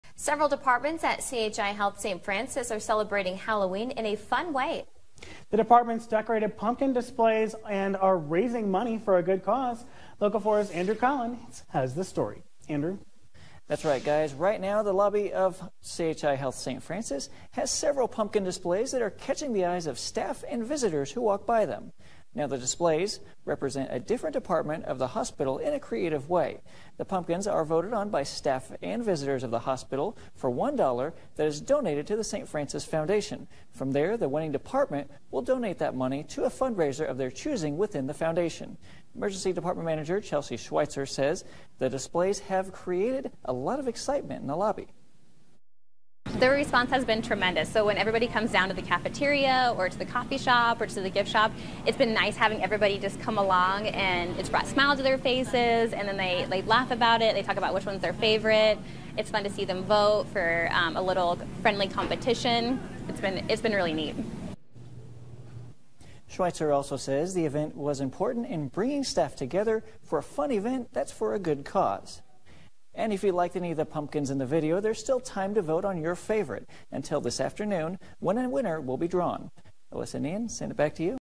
CHI Health St. Francis celebrates Halloween with pumpkin decorating contest
CHI Health St. Francis is celebrating Halloween with a pumpkin decorating contest that supports a good cause.